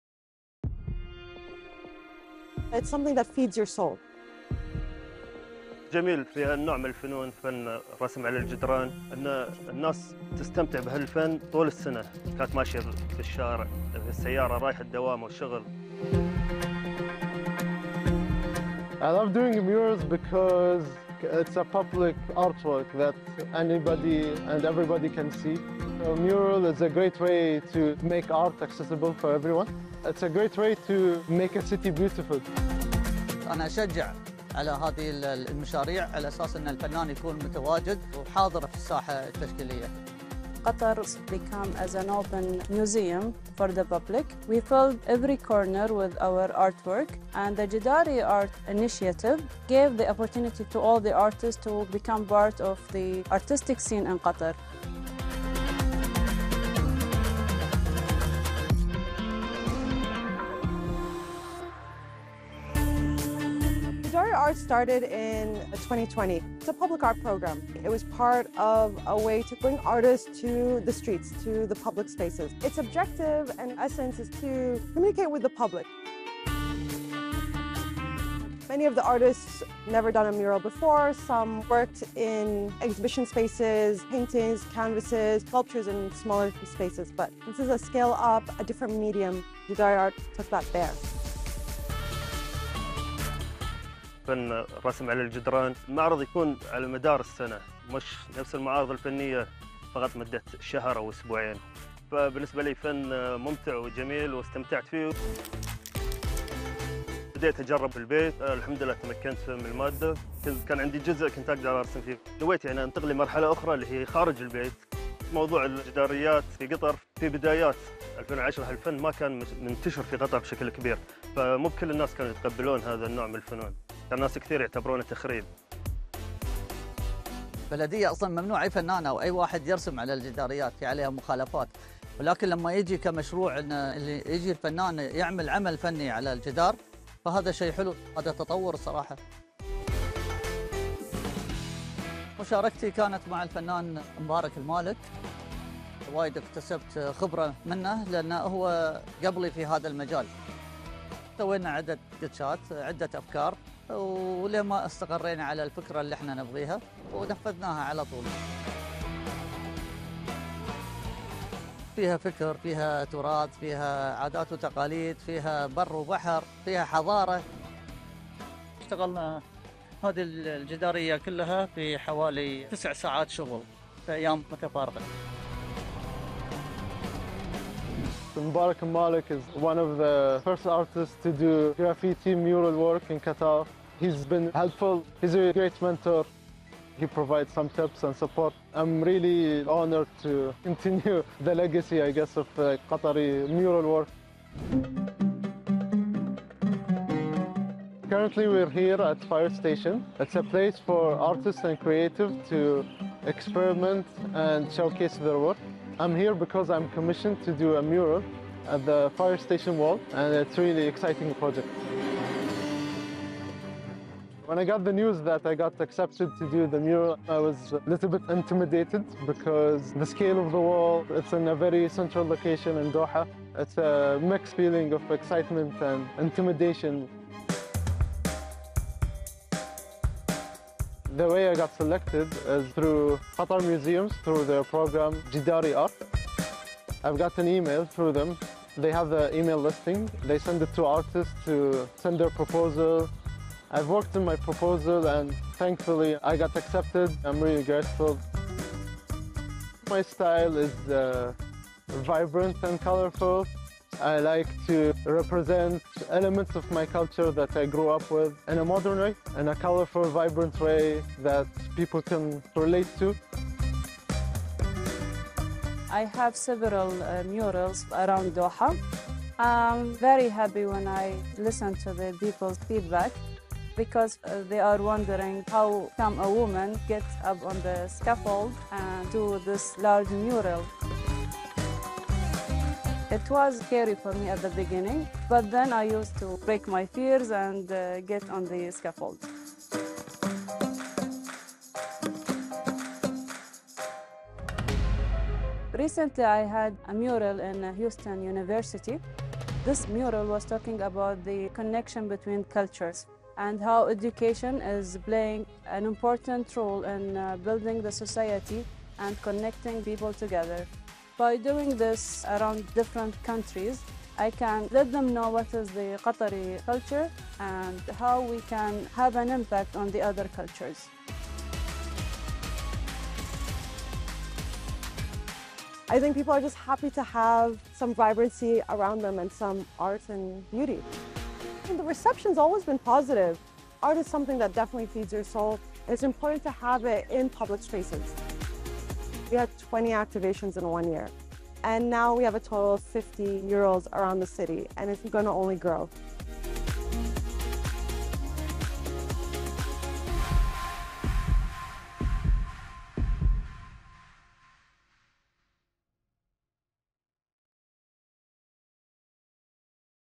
Meet the mural artists adding vibrancy to Qatar’s cityscape | Scenes
"What is beautiful about this type of art, the art of street art painting, is that people enjoy this art throughout the year, they're walking, driving or going to work."

The JEDARIART programme collaborates with talented artists and has breathed new life into Doha's urban landscape